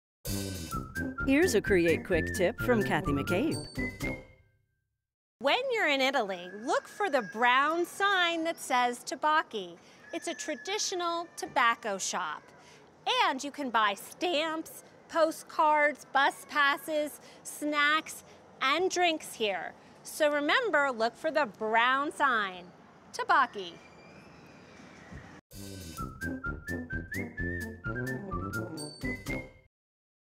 Create Quick Tip: Italian Tabacco Shops
Dream of Italy's Kathy McCabe explains why there's more to Italy's tabacchi shops than meets the eye.